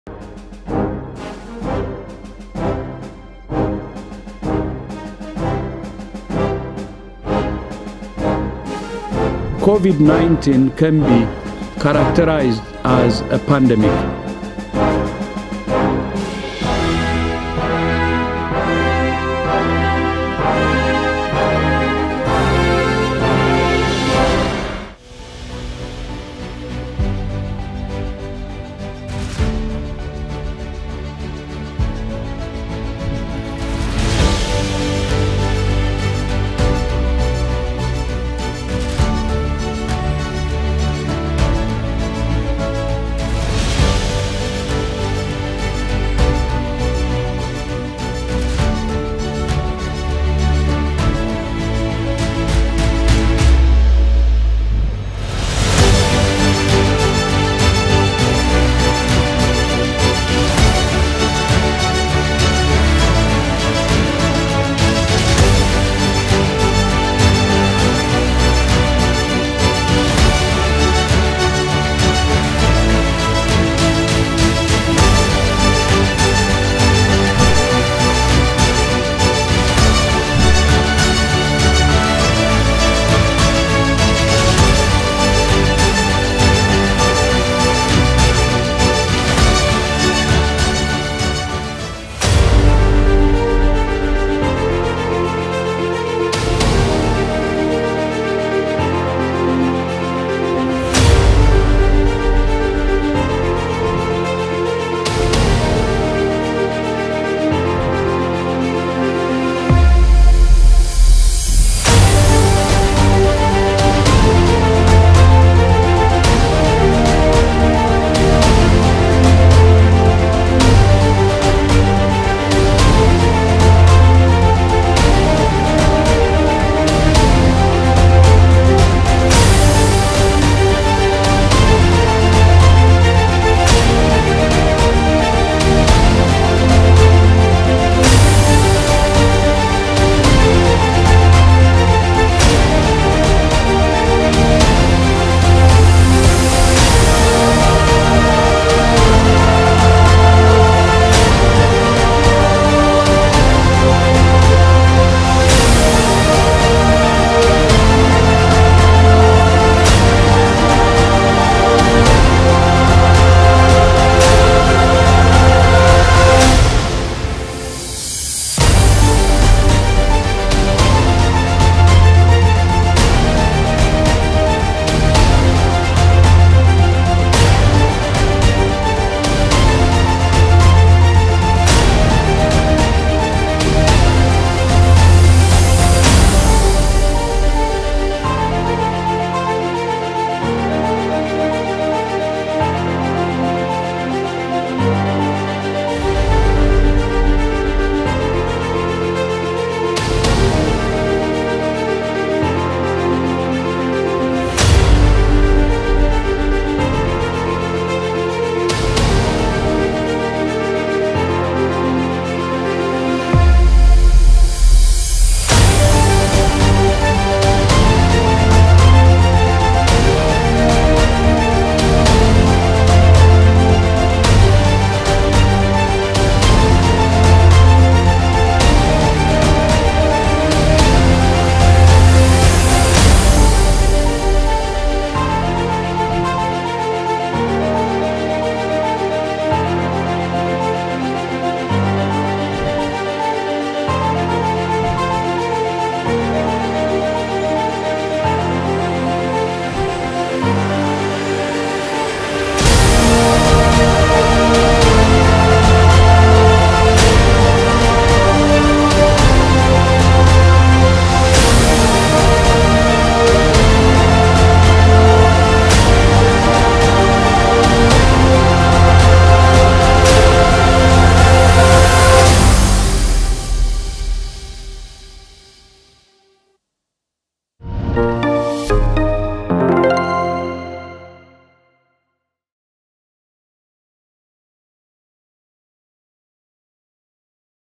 코로나19에 당당히 맞선 대한민국의 과학기술과 ICT
코로나19를 극복하기 위한
대한민국의 과학기술과 ICT!
우리가 가진 그 우수한 DNA를 만나봅니다.